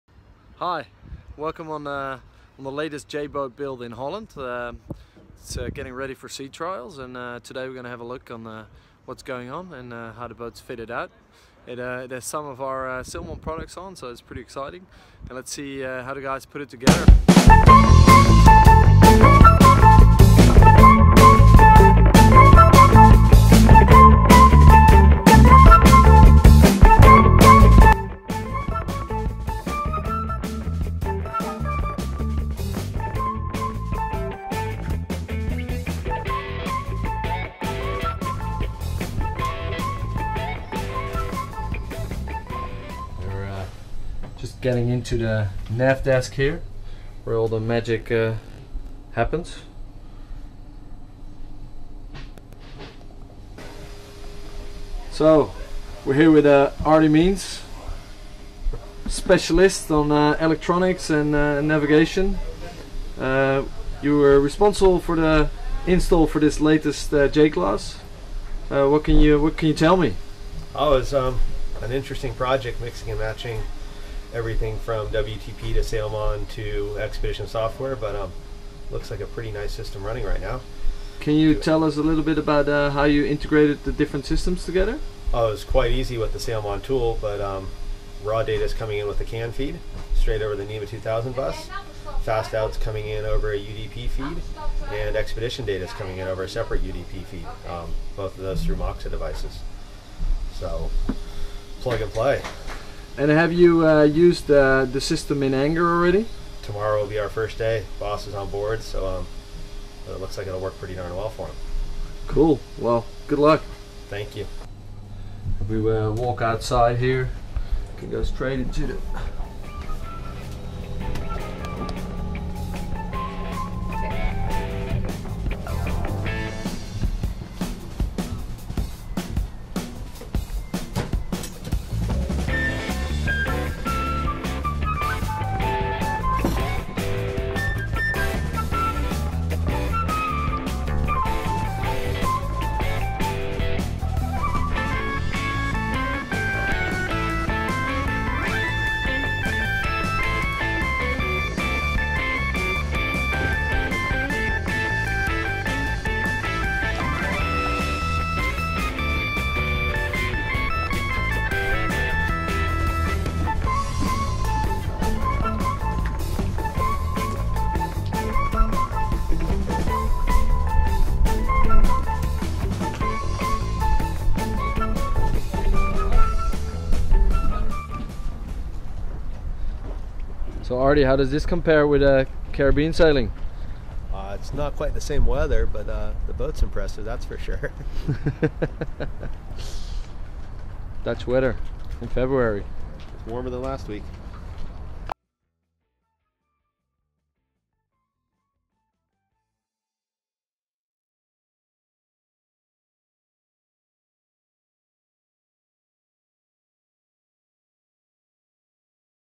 latest J Class
This video is about the latest J class build in the Netherlands